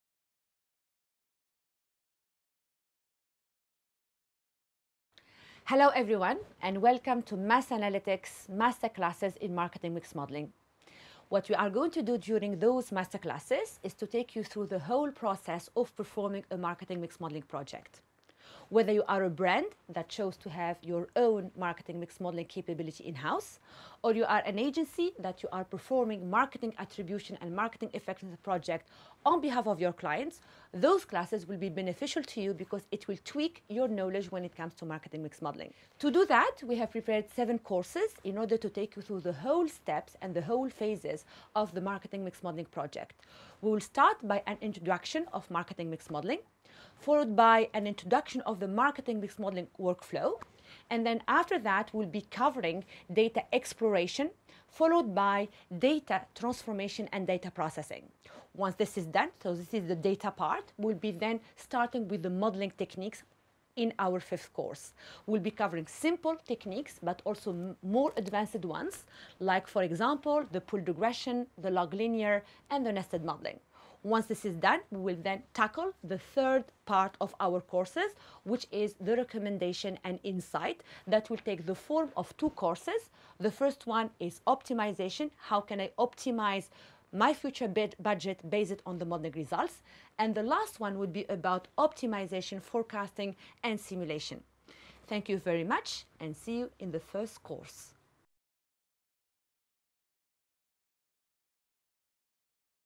Episode 0: Introduction to Marketing Mix Modeling Master Classes
We have designed a comprehensive course on Marketing Mix Modeling composed of eight episodes that cover the full process of delivering a MMM project to help you master the MMM practice.

In each episode we share different perspectives and best practices based on MASS Analytics’ experience running MMM for multiple verticals.

Episode 0 – MMM Master Classes outline:
Episode 1: Introduction to MMM
Episode 2: The MMM Workflow
Episode 3: Data Required in MMM
Episode 4: Data Exploration
Episode 5: Data Processing
Episode 6: Modeling
Episode 7: Optimization
Episode 8: Prediction